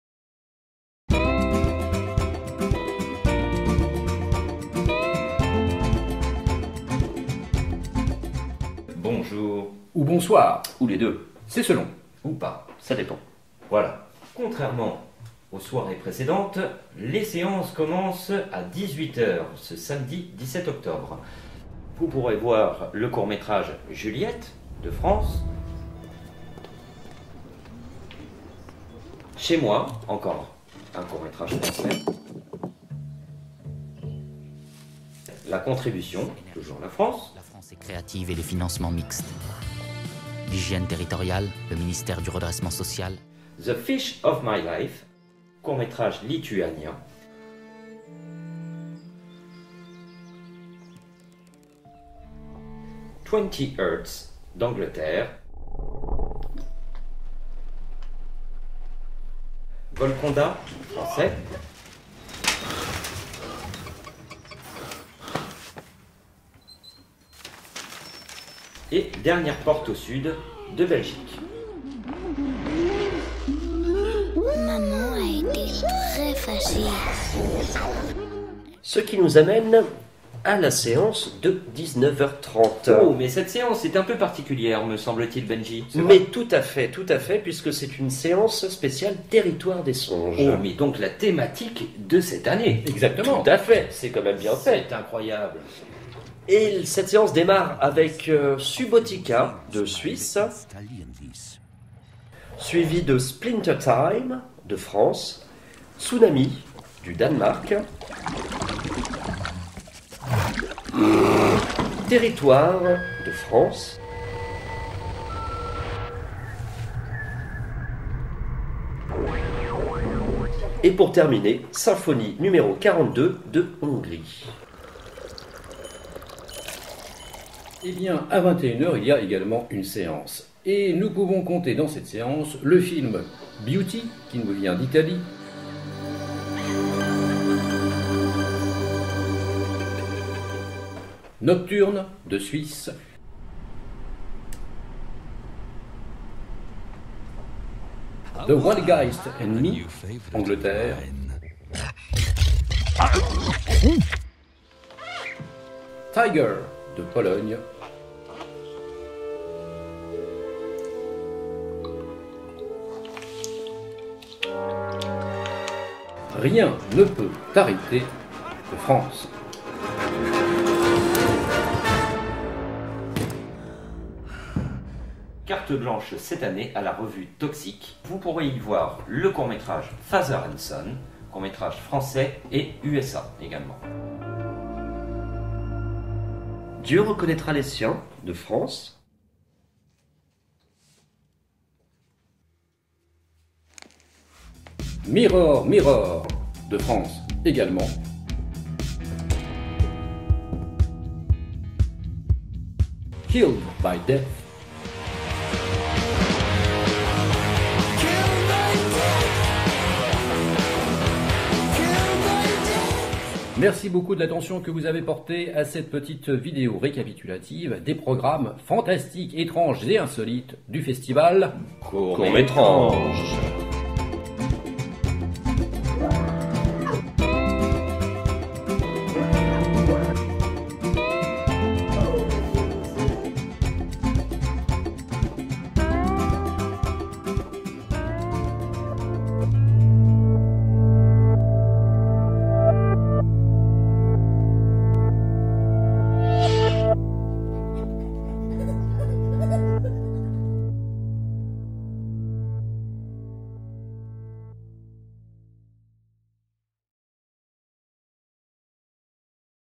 Court Métrange - Samedi 17 octobre 2015
La programmation du festival Court Métrange pour le samedi 17 octobre 2015 !